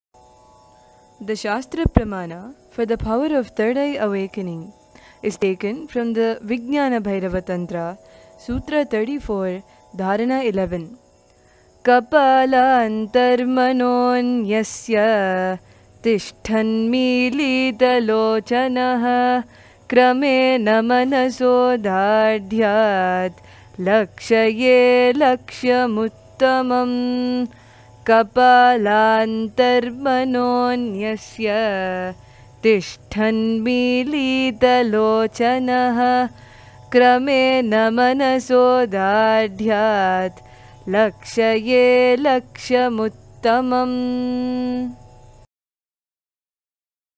Nithyananda Jnana Paddhati Vol 1: TE RV VGBT S34 D11
Research on Children's Extraordinary Brain
Shaastra Pramana: Vijñāna Bhairava Tantra, sutra 34, dhāraṇa 11
Power: Third Eye Awakening/Remote Vision